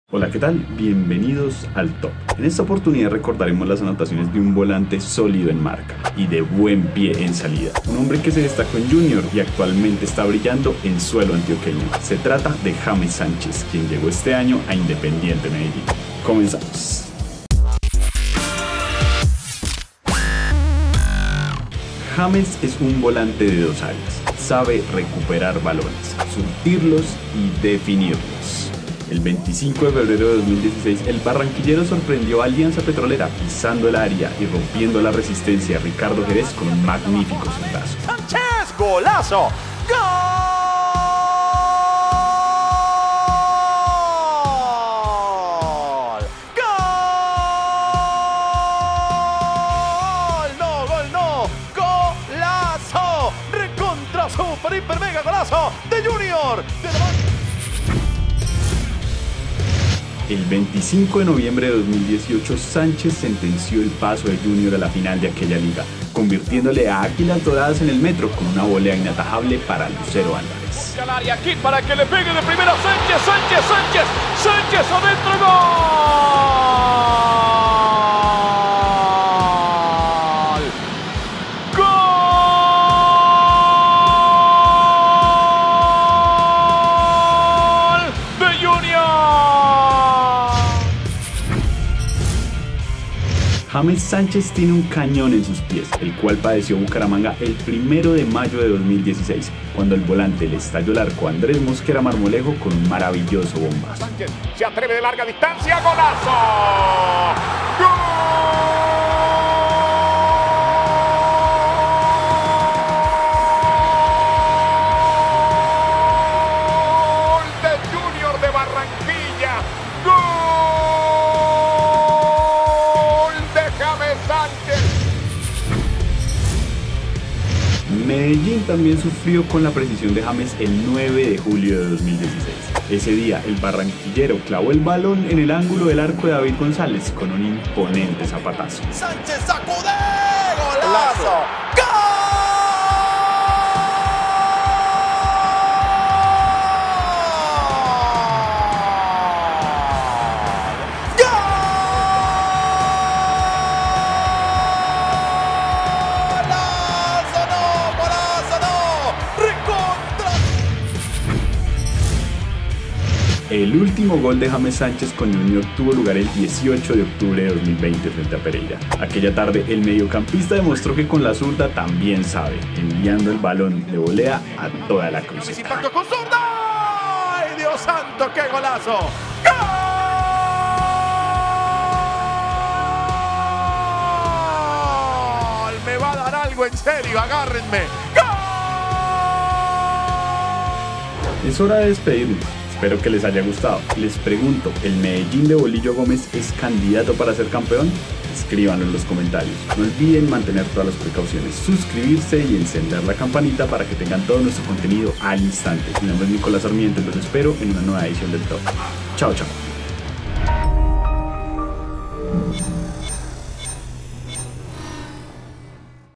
5 goles extraordinarios de James Sánchez con Junior | El Top de Win Sports
El volante dejó el ‘tiburón’ y ahora es figura de Independiente Medellín.